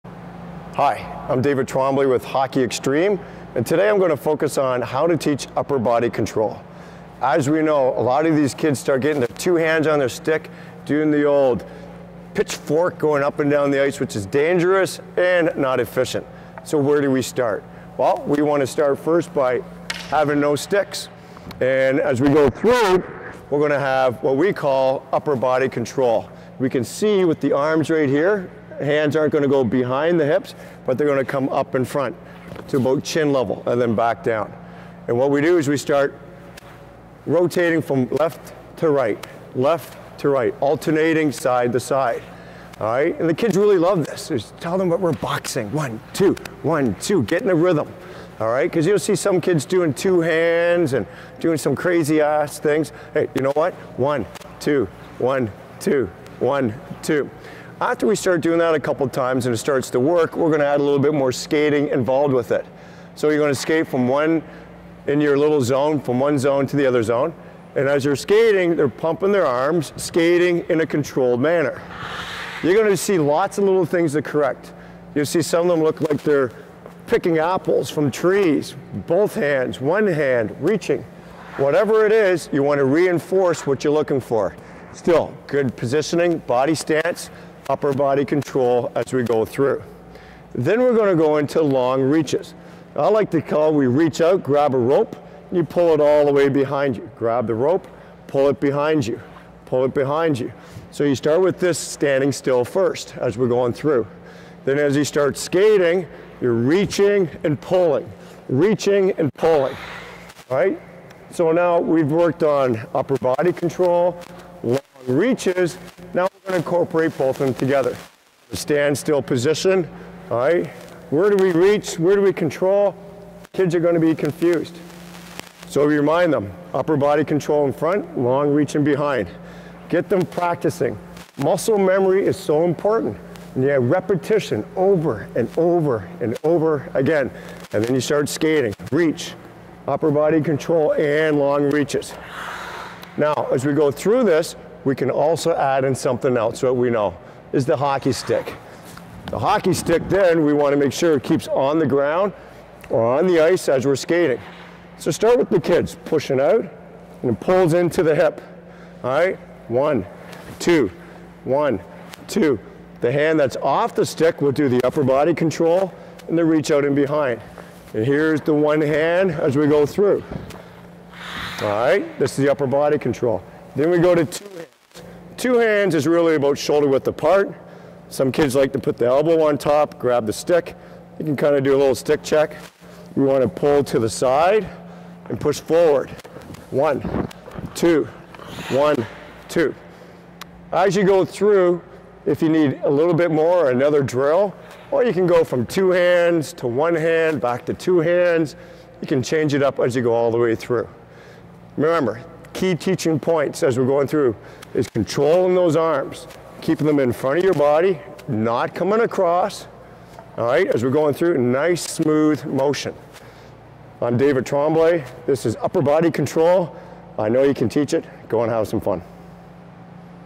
How to teach hockey upper body control
In this video you will learn how to teach players how to keep their upper bodies in control while skating down the ice.  These techniques will assist players in maximizing power and control as they propel themselves around the ice.